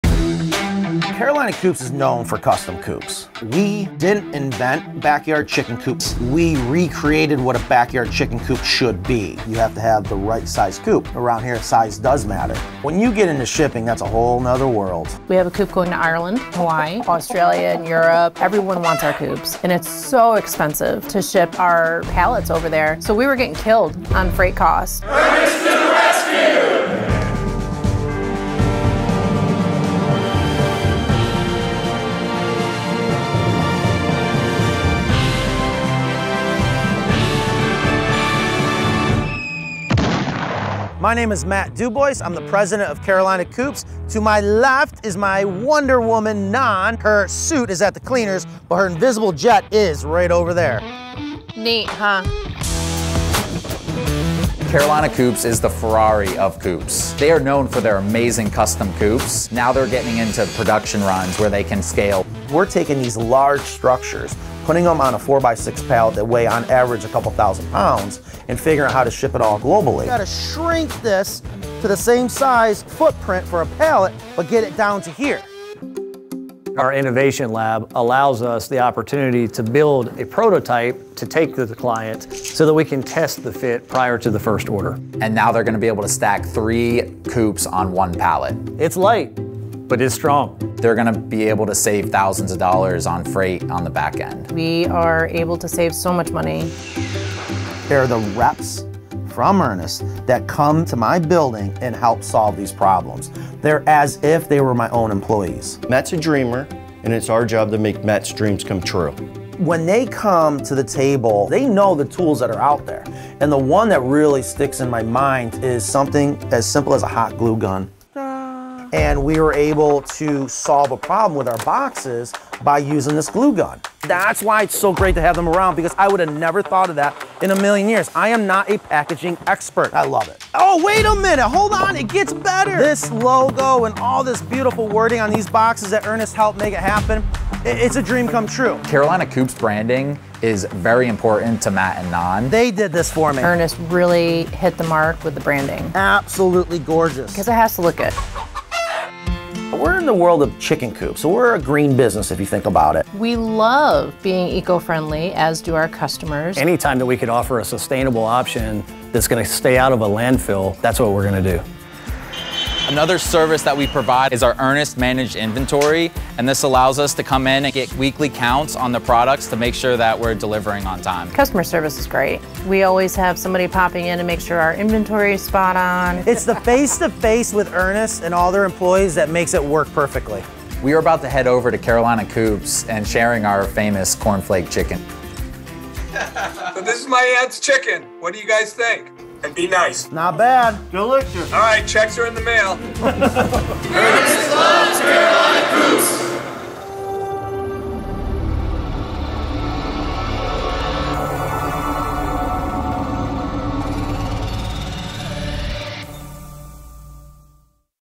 Ernest x Carolina Coops: An egg-cellent partnership.
​@CarolinaCoopsVideos  takes chicken coops to the next level. And they needed their packaging partner to follow suit. Enter Ernest. Discover how the packaging pioneers helped the Ferrari of chicken coops source materials and construct cost-efficient box formats all wrapped in some eco-friendly fun in the video above.